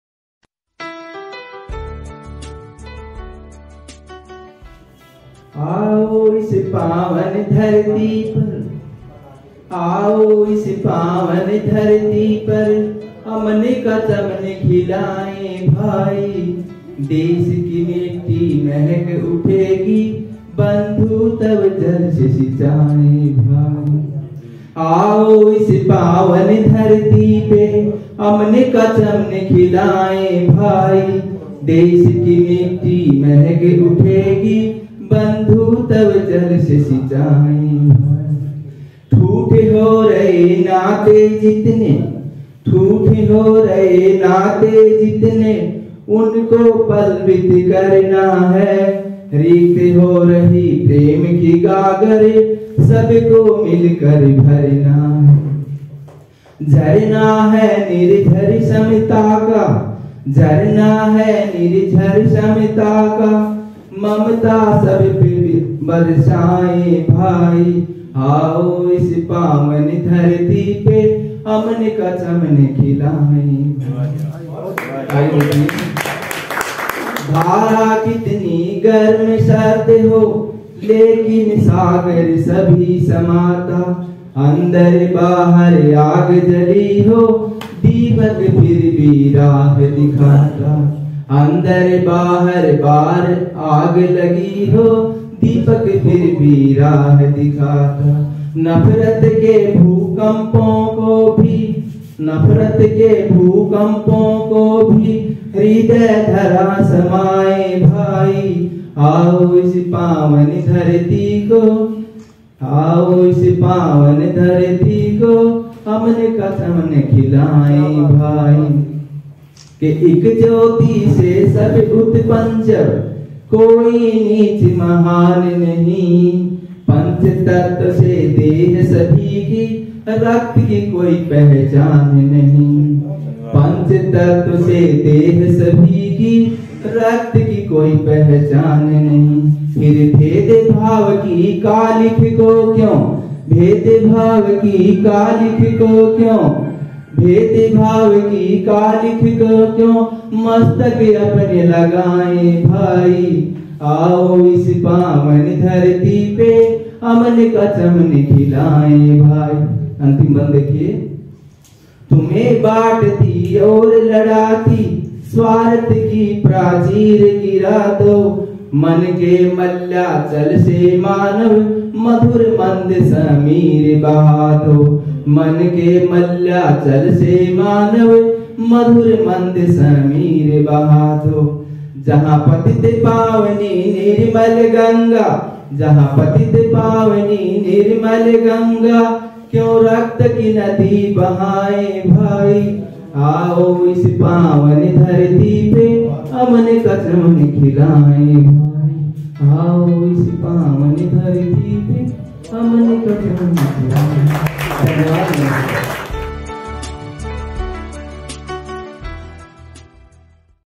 डॉ. चंद्रदत्त शर्मा 'चंद्रकवि' || SagarNama || आओ इस पावन धरती पे अमन का चमन खिलायें भाई
अखिल भारतीय राज साहित्य मंच हिसार (हरियाणा)
31 Oct,2021
बाल भवन जींद

प्रस्तुति : डॉ. चंद्रदत्त शर्मा 'चंद्रकवि'(रोहतक)

Edit by:- Sagarnama You Tube Channel